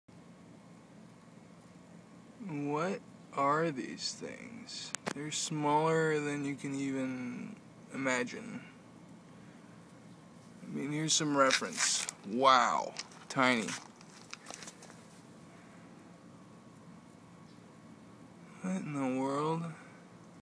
weird bugs
a bunch of little yellow pricks zoomin around.